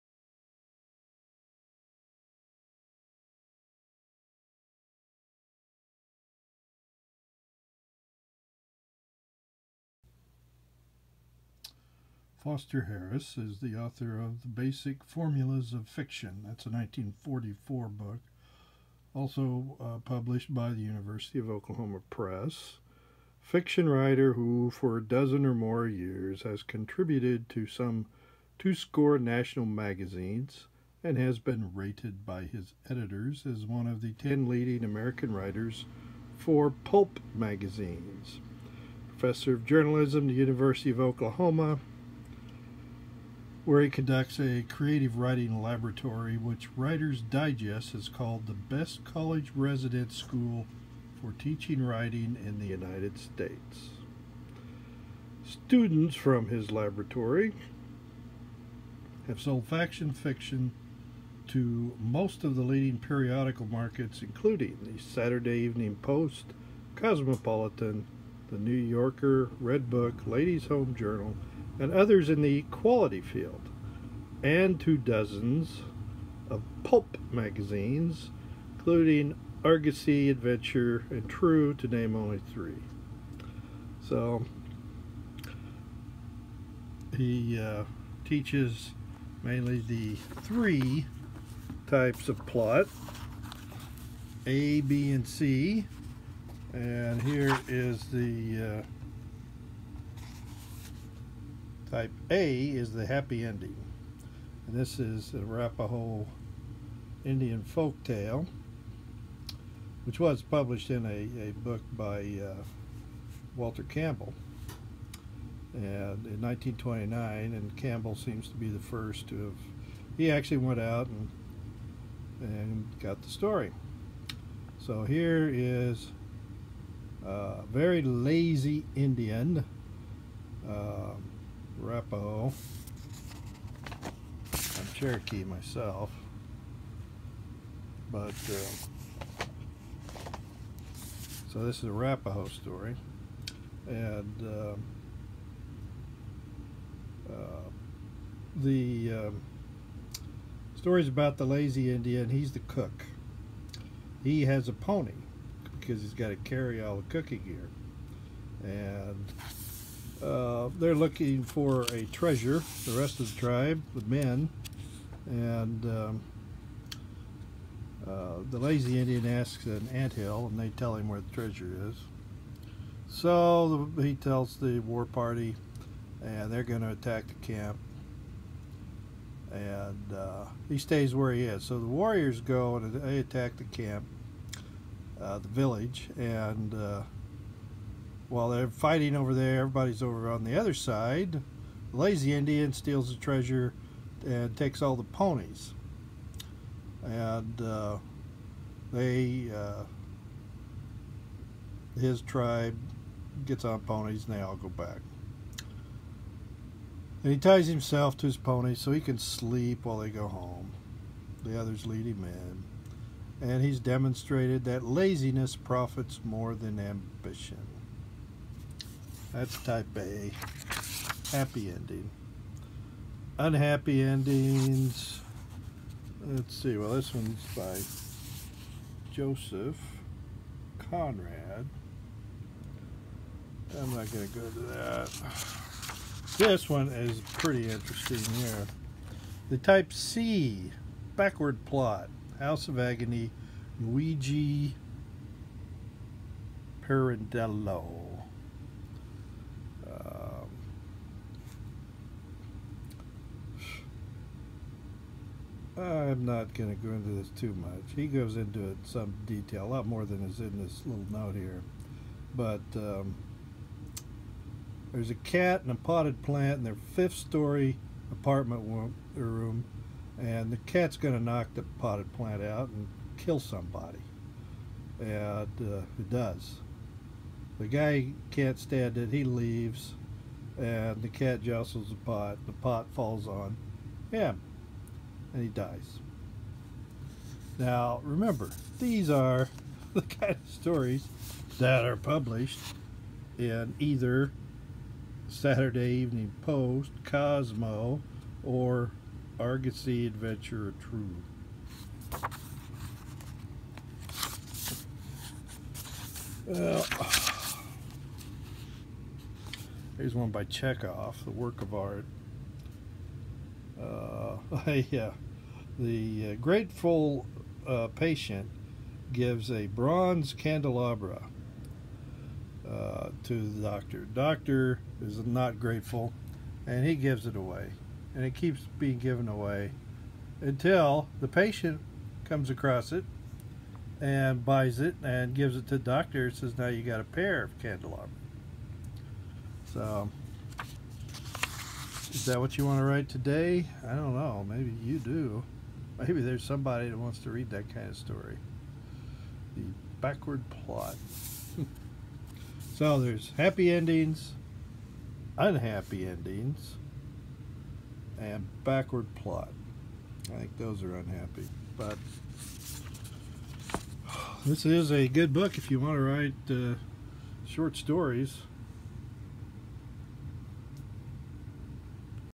Foster Harris 1959
This is primarily about Foster Harris's 1959 book on writing commercial short stories for the market of that day: magazines. Harris was one of the founding directors of the OU course in professional writing. (Other later directors include Walter Campbell, Dwight Swain, Jack Bickham, and Deborah Chester.) THE BASIC PATTERNS OF PLOT according to Harris in this book (again, for magazine short stories) have happy endings, unhappy endings, and the backward plot, which typically is an unhappy ending.